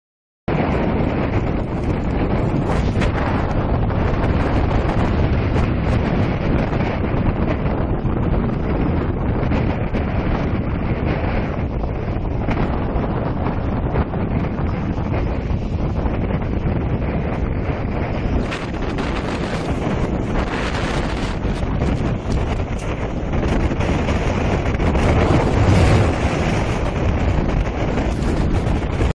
Violenta mareggiata sul lungomare Caracciolo di Napoli: l’acqua su strada e marciapiedi
Danni nel tratto da Piazza Vittoria alla sede Universitaria

Vento forte e mare in tempesta nella tarda serata di lunedì 28 dicembre a Napoli. Il mare in tempesta ha invaso il lungomare Caracciolo causando numerosi danni ai locali della zona. ( Corriere Tv ). Guarda il video su Corriere